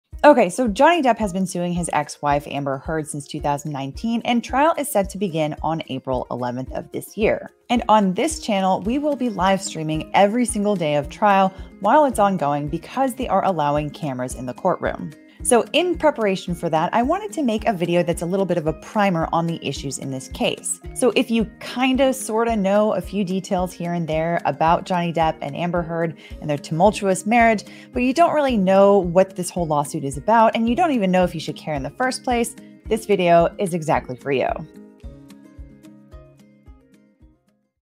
Johnny Depp V. Amber Heard | New LegalBytes Video This Afternoon!
Hello all! Just a short teaser for a new video that will drop on the main channel this afternoon giving an overview of the Johnny Depp V. Amber Heard Defamation Lawsuit stemming from her comments in a Washington Post Op-ed. We'll update the description here with a link once it goes live.

Support content like this here!